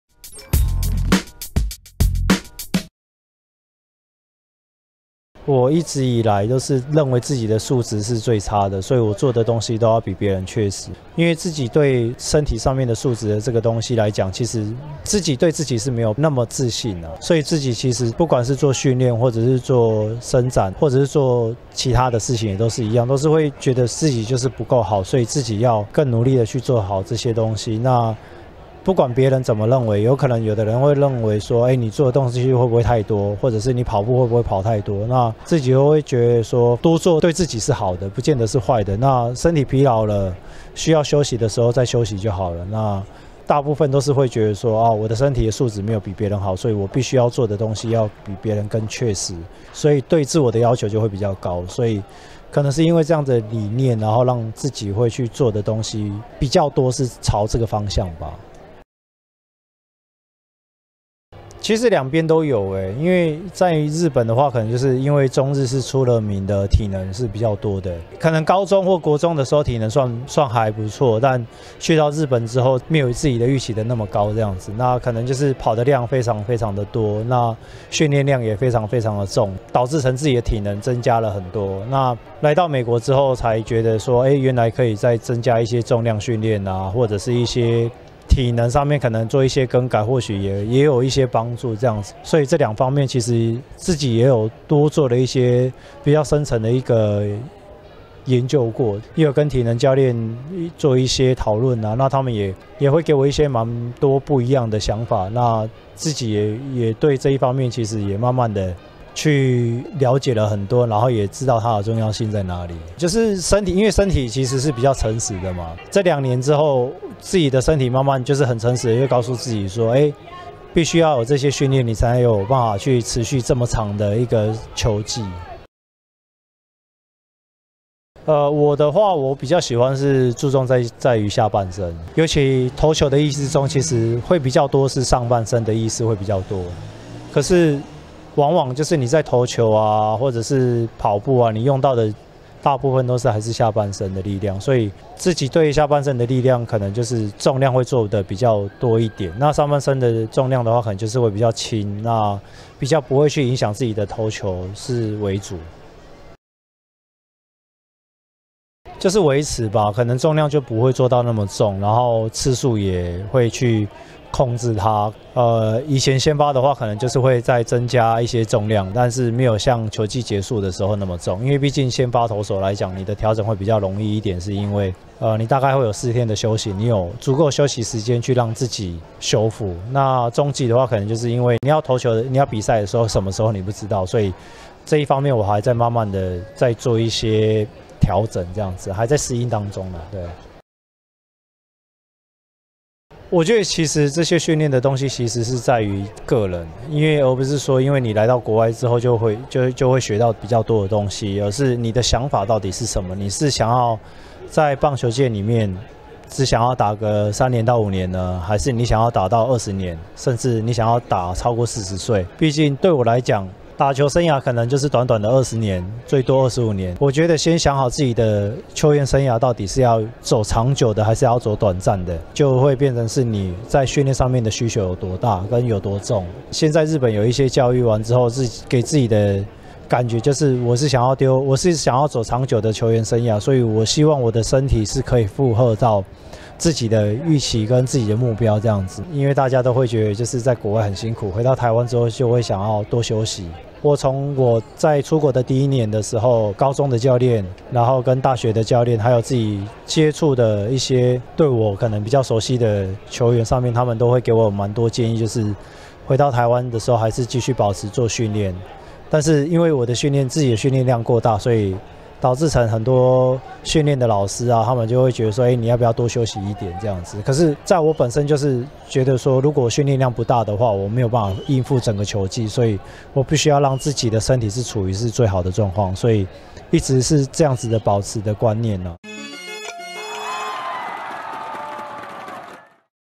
【MLB】陳偉殷的訓練人生 堅持造就成功
是怎麼樣的的信念，讓旅外多年的陳偉殷如此堅持與鑽研在自主訓練上呢？

就讓人稱「訓練狂」的他，親自告訴您～

@陳偉殷 チェン・ウェイン CHEN WEIYIN

★TSNA Instagram 在這兒 Follow起來!